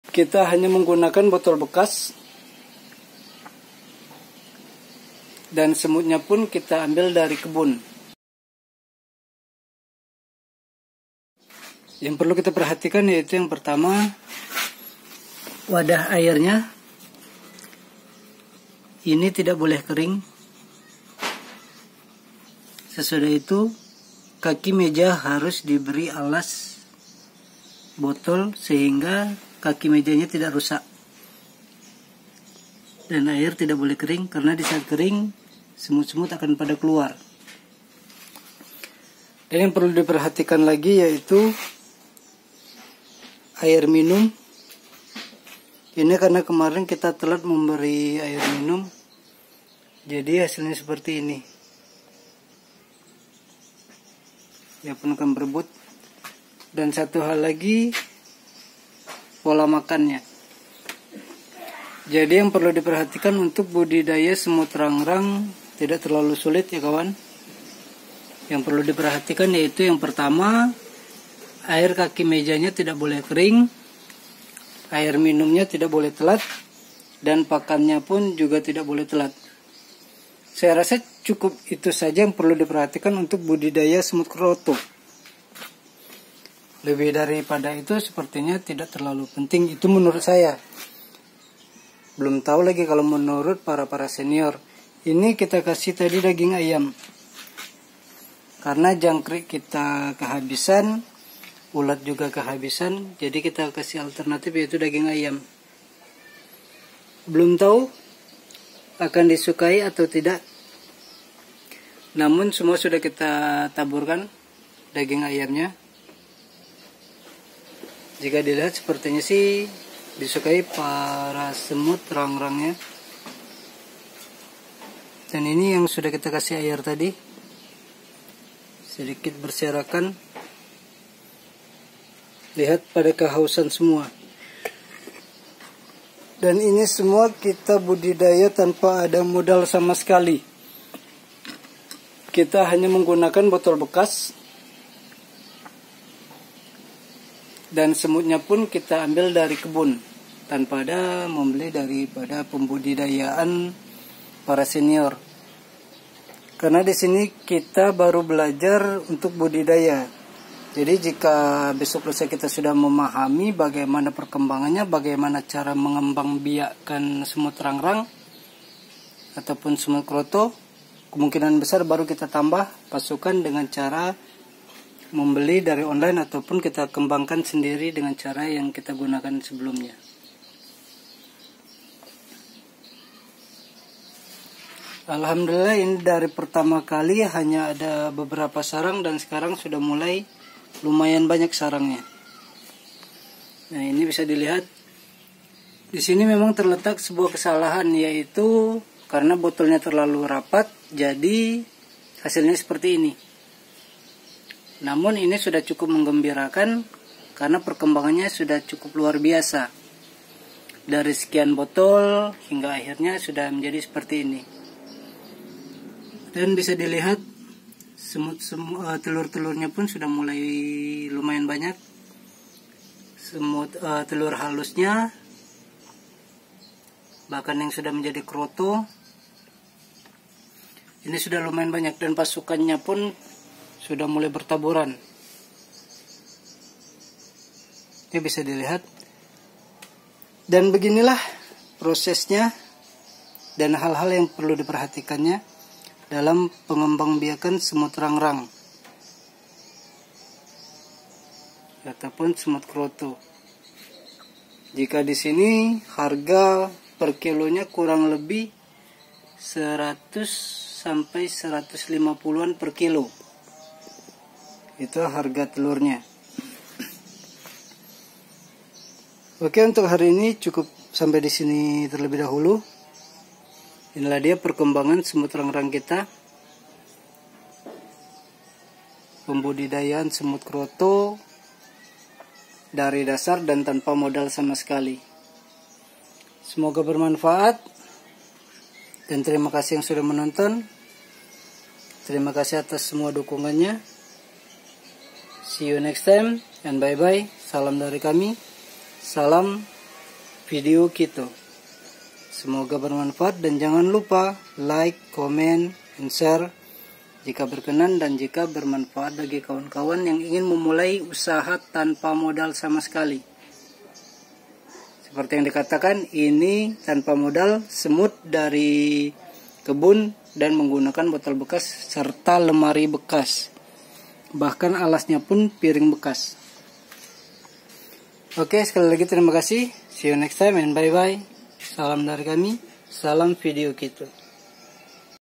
Pemula kroto, Belajar Budidaya semut Kroto
Hari ini kita membahas hal yang perlu diperhatikan dalam pembudidayaan semut kroto
Semoga bermanfaat